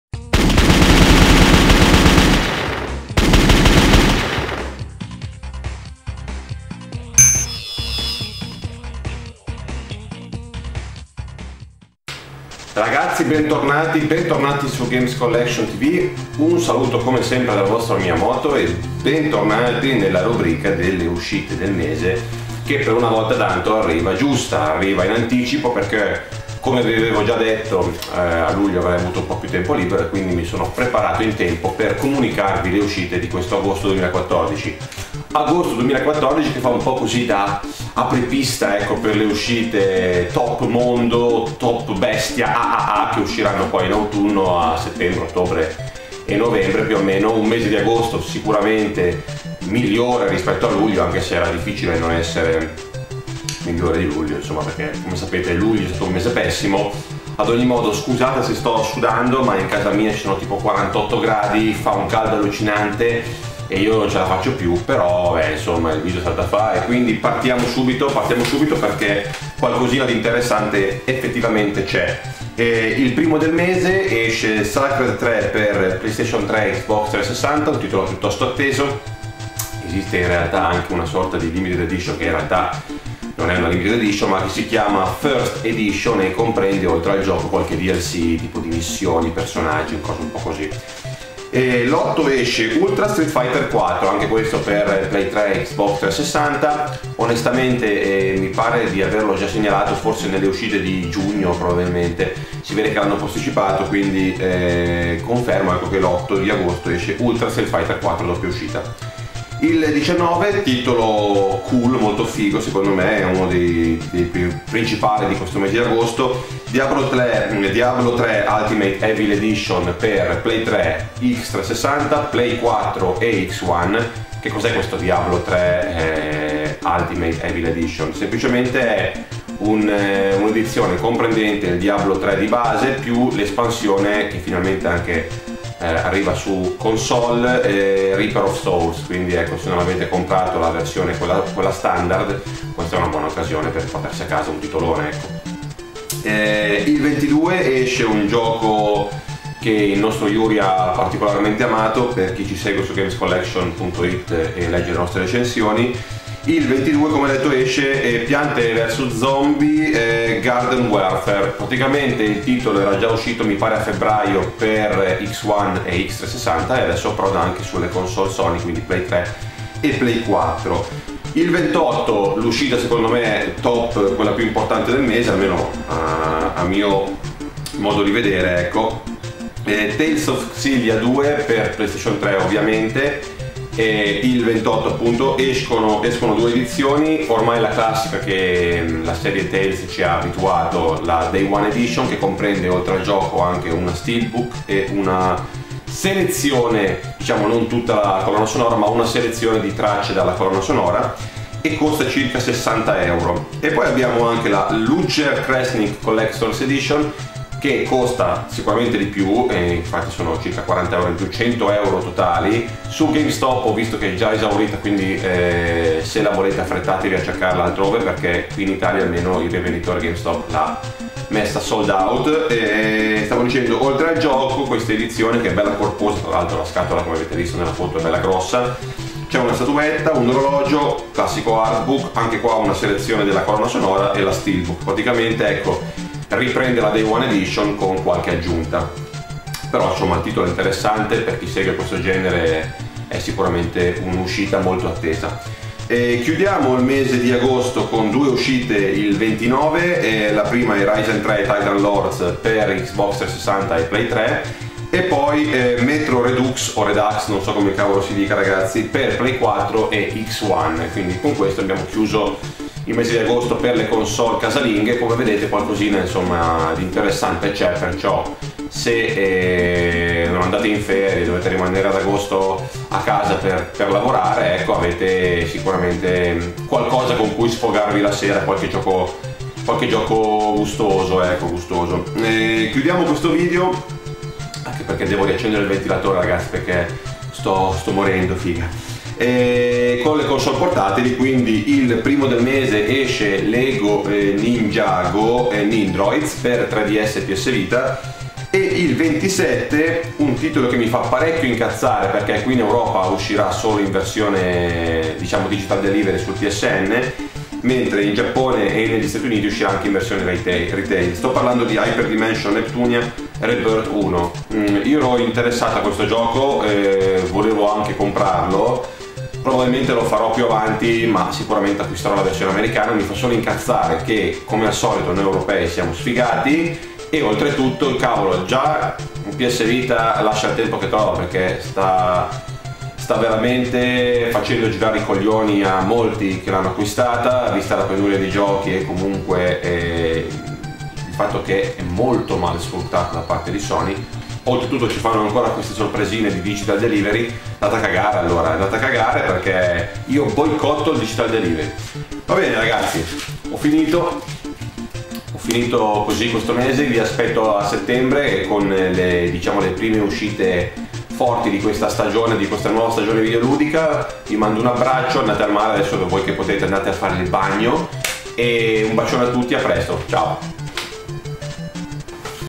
Uscite Videoludiche Agosto 2014 By Miyamoto - PS3 / PS4 / XBOX 360 / XONE / 3DS / PSVITA
Incredibile ma vero!! Il nostro Miyamoto è riuscito a consegnare un video secondo la scaletta, anzi, addirittura con qualche giorno di anticipo!! Scopriamo insieme quindi le uscite di questo caldo mese di agosto...un mese che fa da anticamera all'autunno, il periodo preferito da tutti i videogiocatori!!

Buona visione!

Video a cura di Vittorio "Miyamoto" Spagarino
Montaggio a cura di Alex "Grendizer73" Badella
- La più grande community italiana di collezionisti e appassionati di cultura del videogioco!